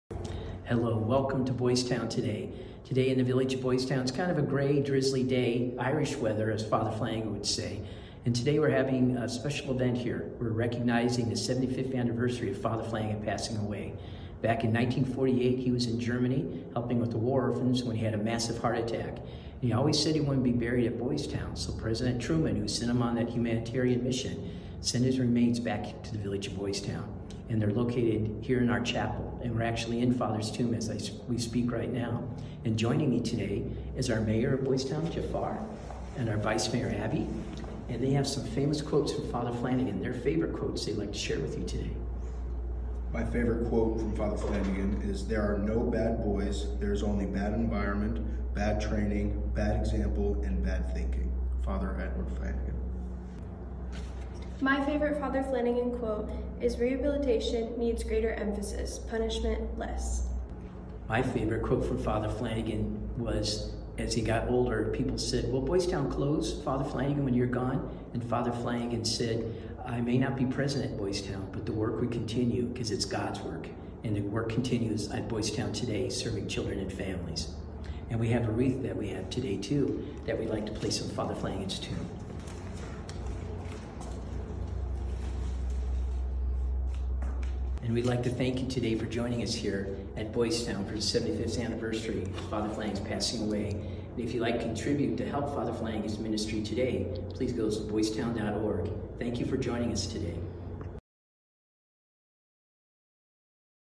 Honoring Father Flanagan's Death on the 75th Anniversary
Today we honor our founder, Father Edward Flanagan, on the 75th anniversary of his death. The news was flashed around the world; residents of Boys Town heard of their leader’s death from a radio bulletin.
The boys of the home immediately gathered in the chapel to pray for Father Flanagan. President Truman sent the message: “He has left a living monument in the countless boys who are today honest men and upright citizens because of his benign influence in the inherent goodness of human nature.” From all over the world, letters and telegrams of condolence and sympathy flooded the home, many addressed to the boys who had lost their “father.” Boys Town alumni from all parts of America returned “home” to say goodbye to the beloved priest. Due to the large number of mourners who converged on Boys Town, two Masses were conducted in Dowd Memorial Chapel, where Father Flanagan’s remains were laid to rest in the former baptistery, where they still are today. Join us as we reflect on some of our favorite quotes from Father Flanagan and lay a wreath on his tomb. 💚